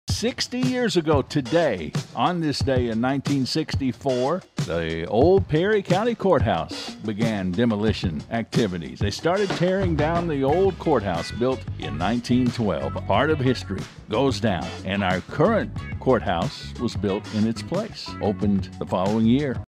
WSGS History: July 8, 1964 - workers began tearing down the old Perry County Courthouse.
WSGS History: 60 years ago today, July 8, 1964 - workers began tearing down the old Perry County Courthouse that was built in 1912